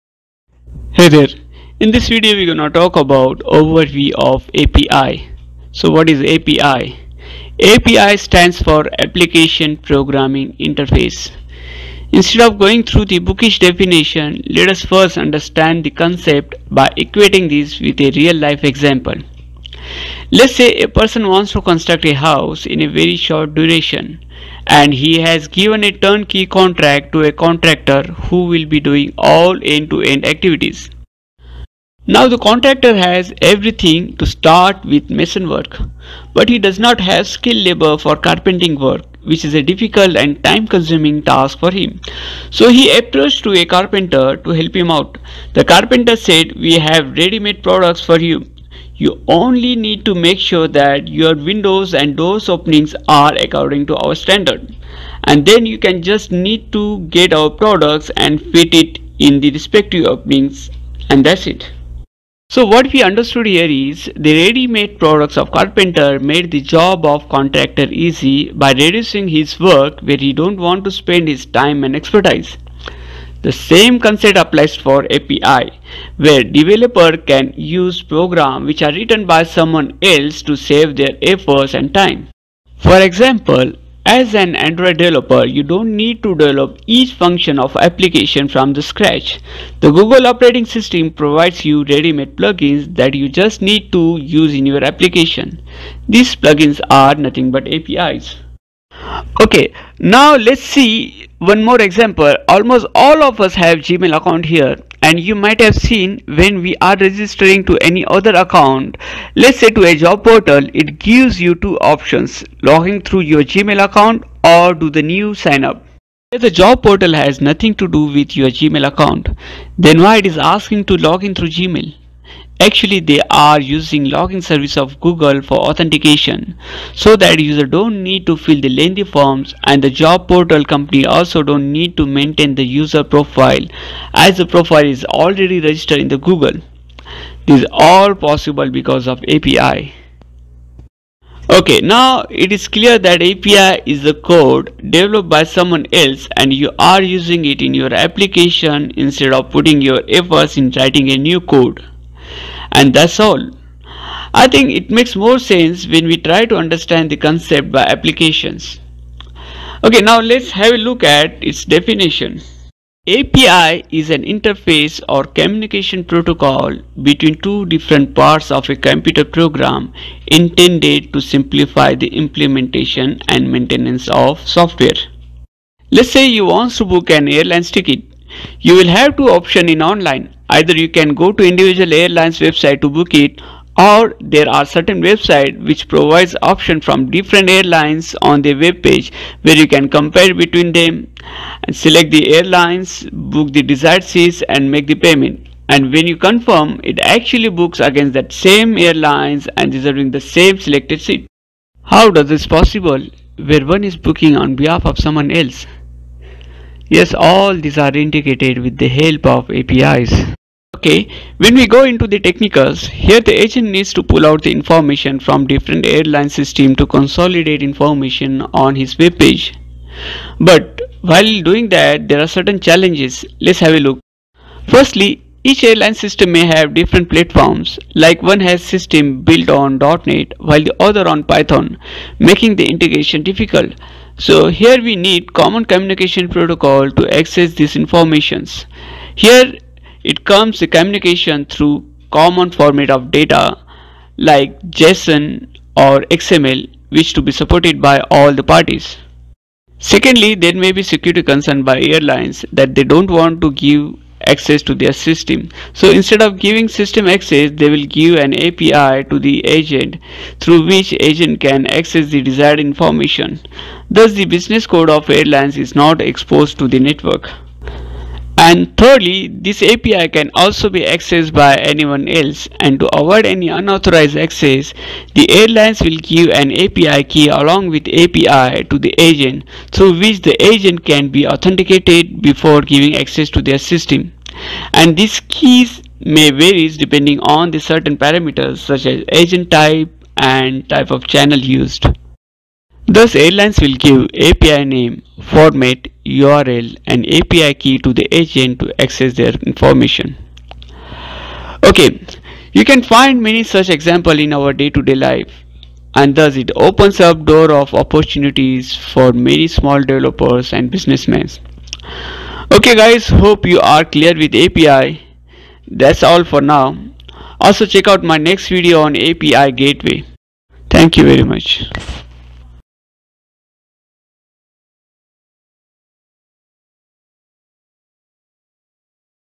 API Overview
Let’s start with the basics of API. In this video, an attempt has been made to explain the API in layman terms so that any person can understand and relate it with its realtime application. An Application Programming Interface (API) is nothing but a common set of routines, protocols or programs used for data transmission between one software product to others. It has many functions usually it is used to speed up software development by integrating it between different systems. To understand it let us take a familiar example where a man is constructing his house and there are many components such as windows, doors, or nowadays we get precast materials for partitions and flooring which contractor doesn’t need to cast on site. He can simply get it from the manufacturer and install it on the building to speed up the construction work. The API serves the same purpose where one developer can use already used and tested set of programs of another developer. The APIs are very critical not only in programming points but also in business terms, which has explained in this video with an airline booking example. It also opened the door for small developers and also there are opportunities for small businesses to develop their websites by integrating with different B2B services.